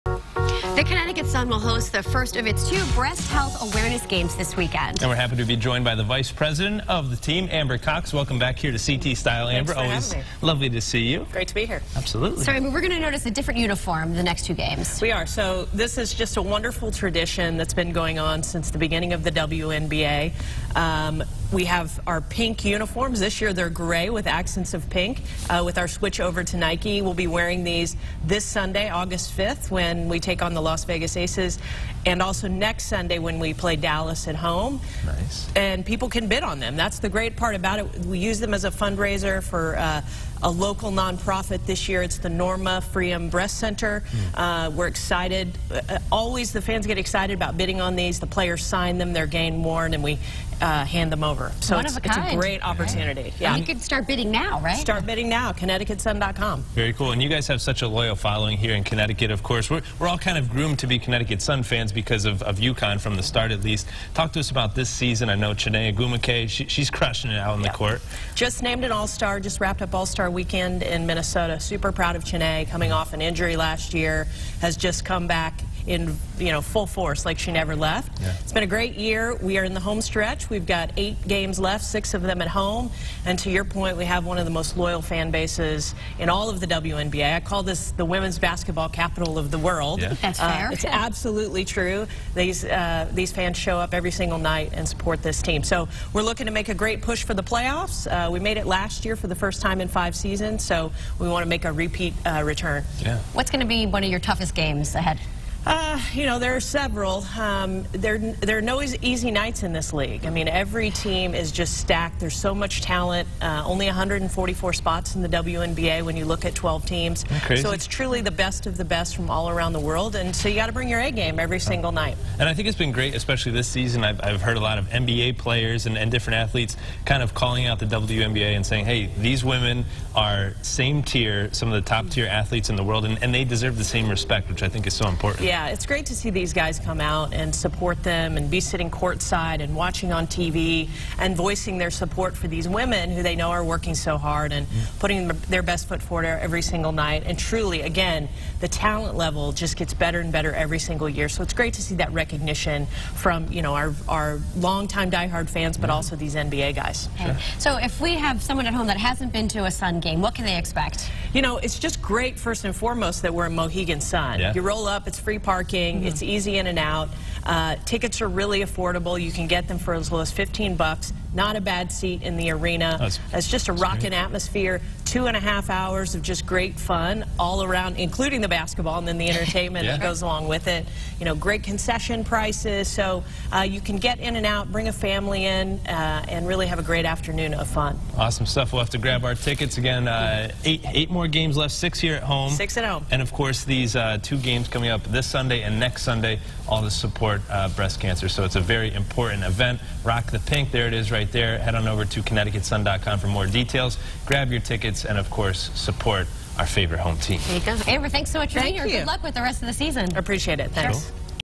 Connecticut Sun bringing awareness to breast cancer
The Connecticut Sun will host the first of it's two Breast Health Awareness games this weekend.